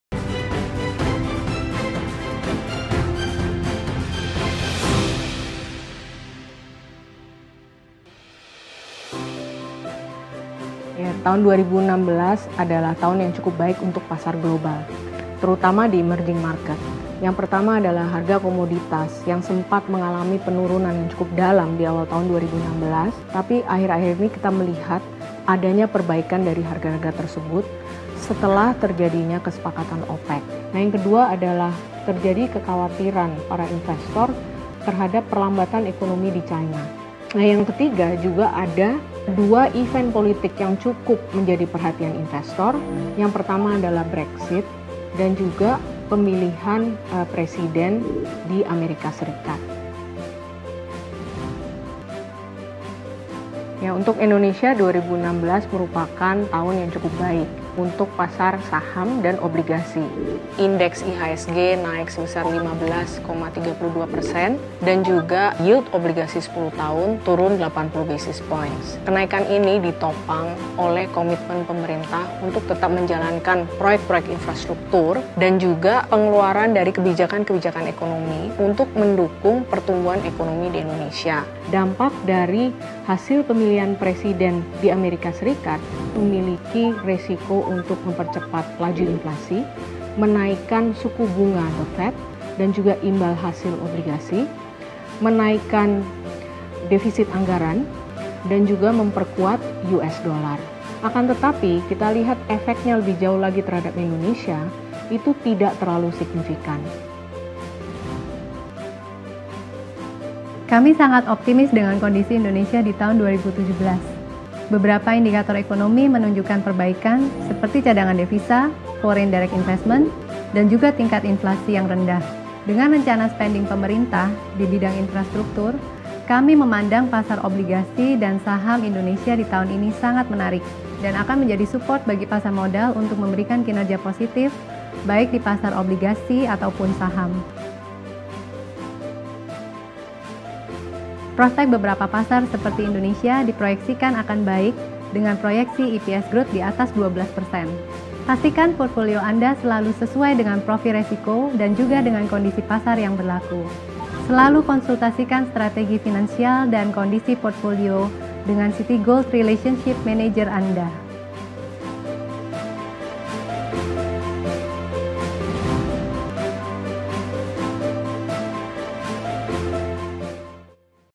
Citi Investment Series, Q1 Market Outlook
Find out more on 2017 market analysis and potential in Indonesia.